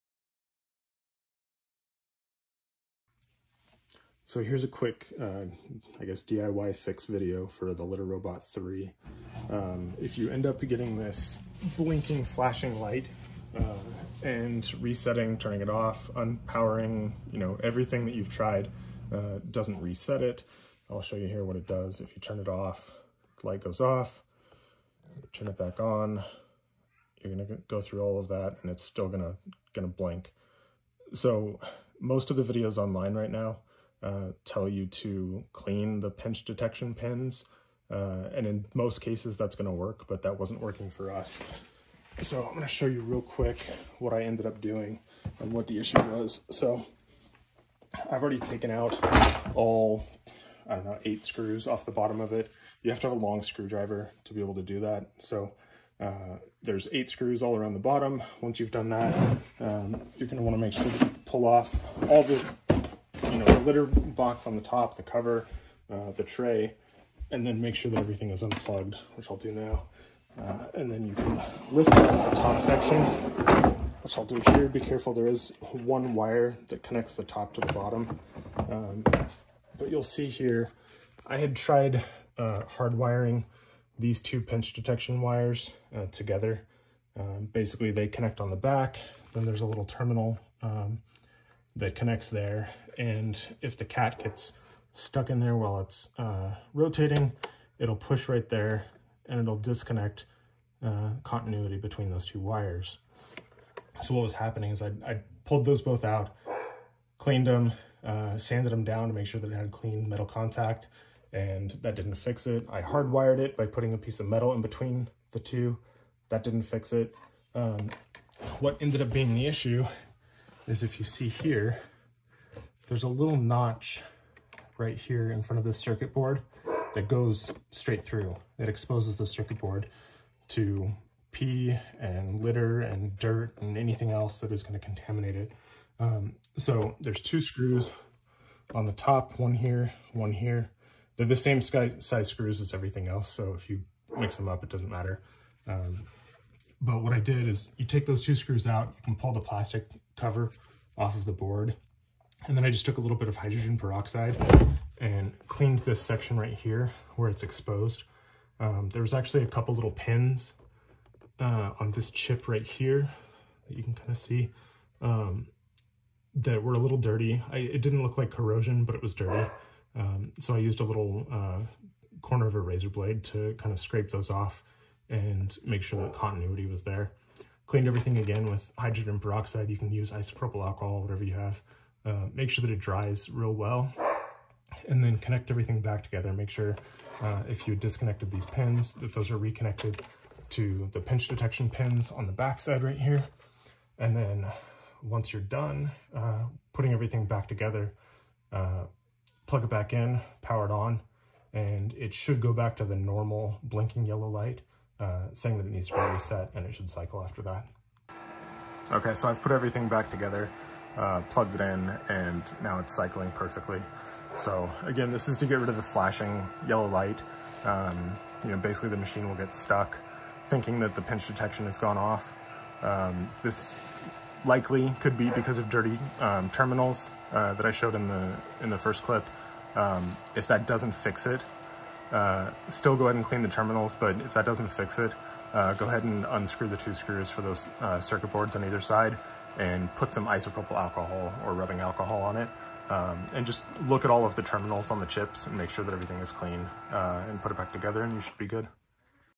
Litter Robot 3 - Fixing Pinch Detection Error
Here’s a quick DIY instructional video to fix the flashing yellow light error that is caused by the pinch detection sensor being stuck, having broken continuity, or a dirty circuit board.